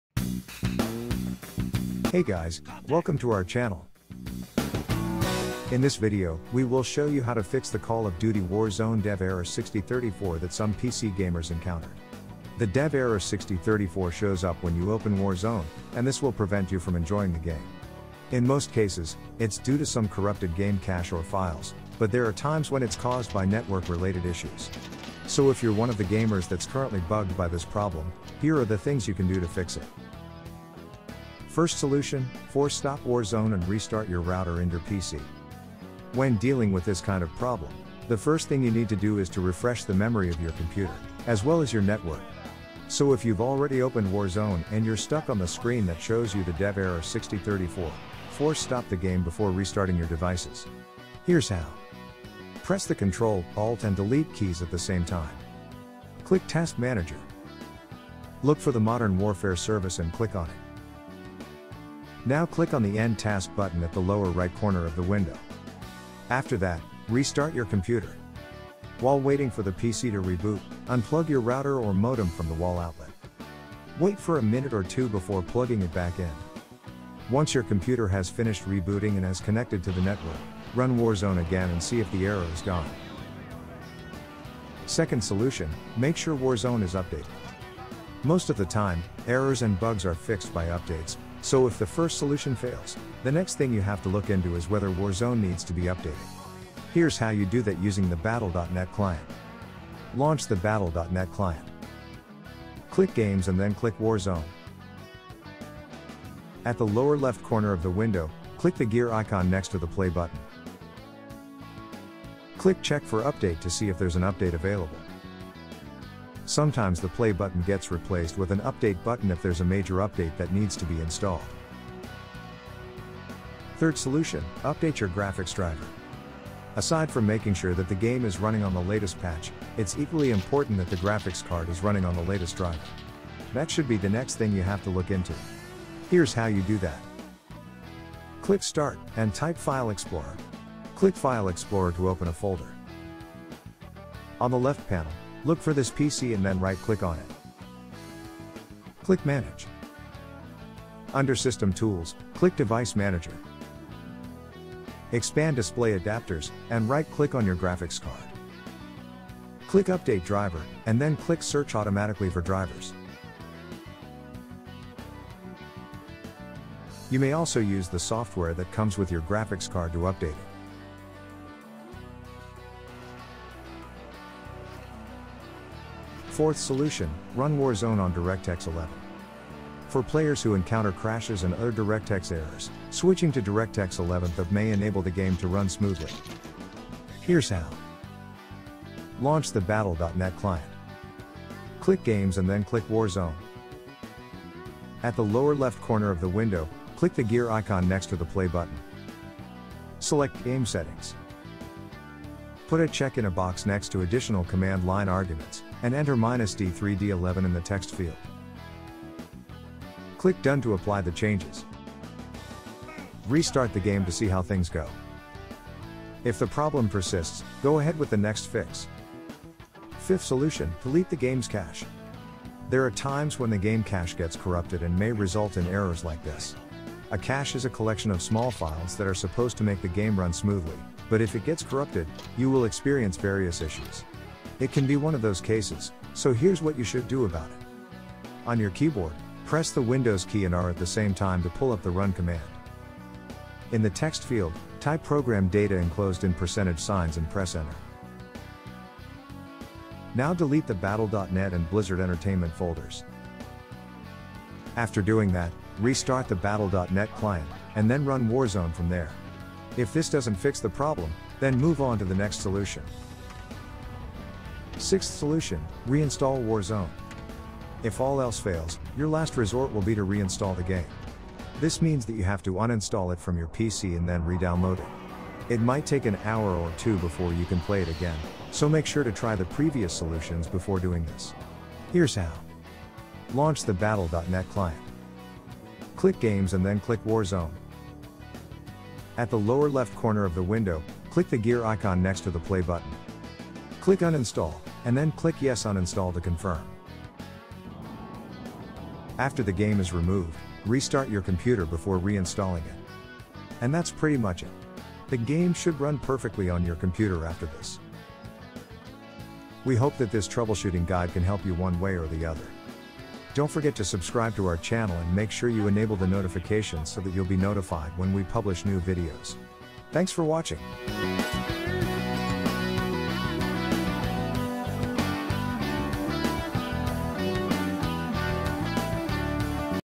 How To Fix The Call of Duty Warzone Dev Error 6328 on PC (Battle.net)
In this video, we will show you how to fix the Call of Duty Warzone Dev Error 63 28 that some PC gamers encountered.

The Dev Error 63 28 shows up when you open Warzone, and this will prevent you from gaming normally. Sometimes, it will also cause the game to crash, and many have already experienced it. In fact, it’s one of the most common errors you may encounter every now and then. So if you’re one of the gamers that’s currently bugged by this problem, here are the things you can do to fix it:

0:00 Introduction
0:32 First solution: Force stop Warzone and restart your router and your PC
1:28 Second solution: Make sure Warzone is updated
2:03 Third solution: Update your graphics driver
2:58 Fourth solution: Run Warzone on DirectX 11
3:41 Fifth solution: Delete the game’s cache
4:24 Sixth solution: Reinstall Warzone

We hope that this troubleshooting guide can help you one way or the other.